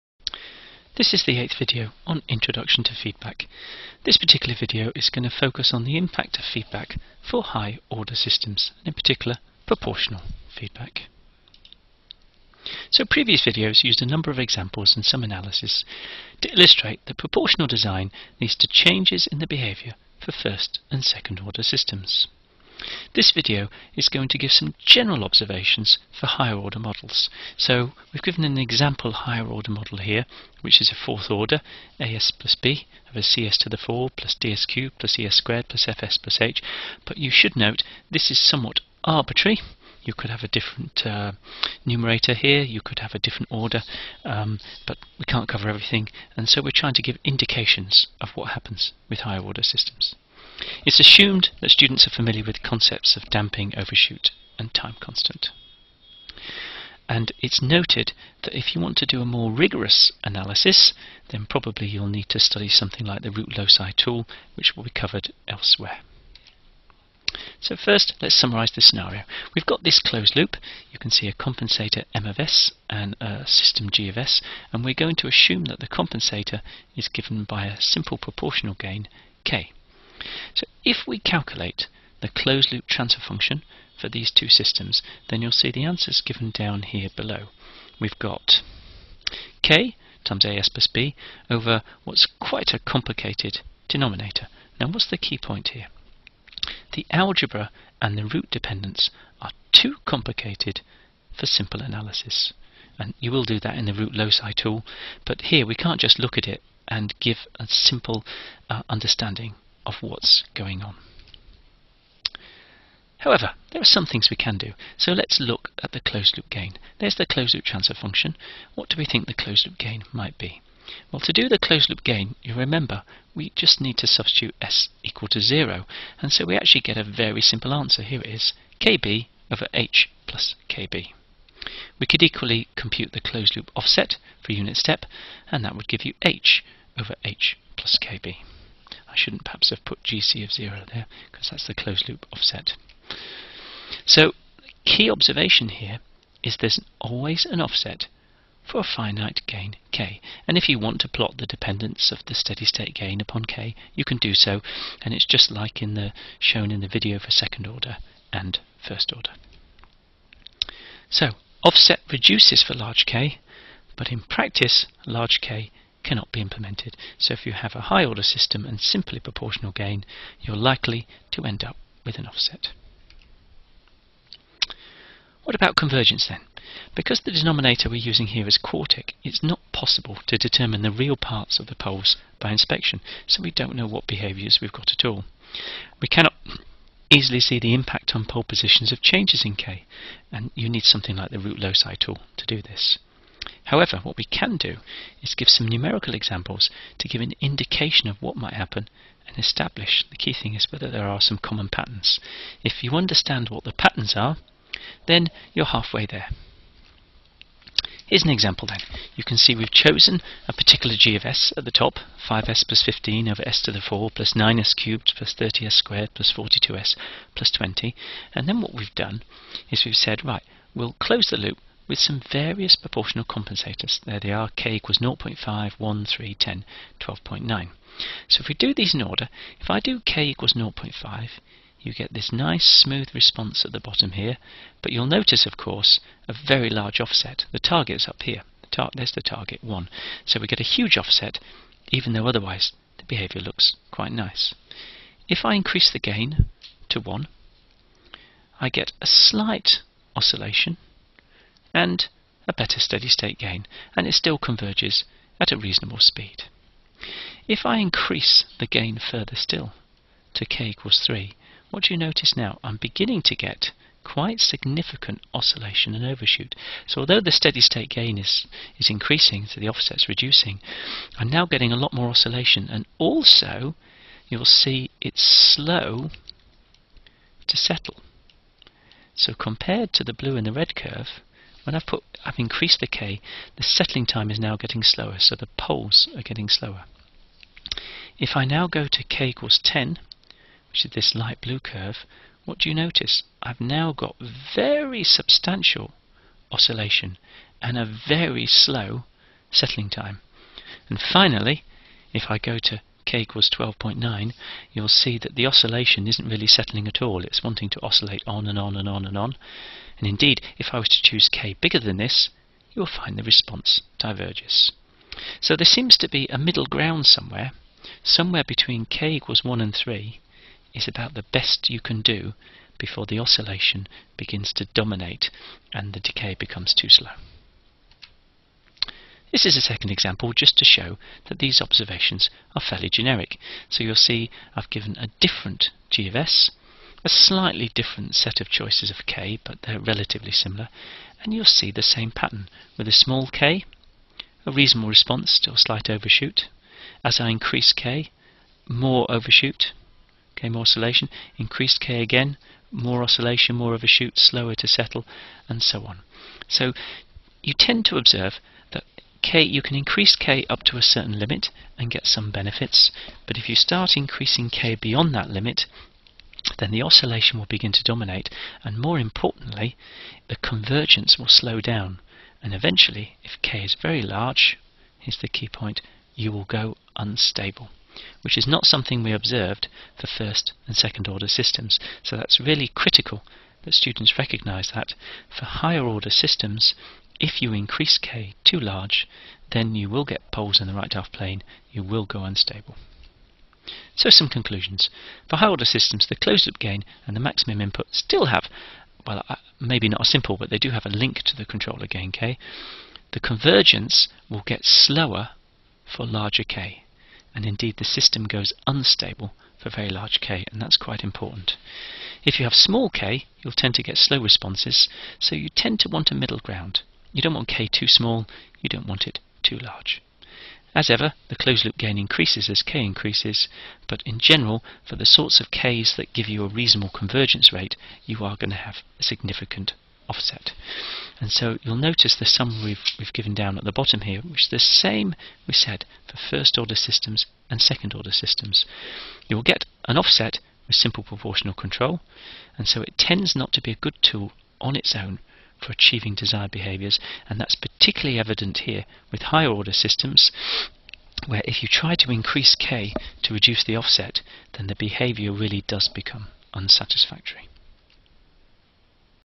Introduction to feedback 8 - quantifying impact of feedback for high order systems
This set of videos introduces feedback concepts and demonstrates how feedback design has a huge and important impact on the world around us. The eighth  video continues the focus on  quantifying  the effect of proportional feedback on behaviours and here focuses on high order systems.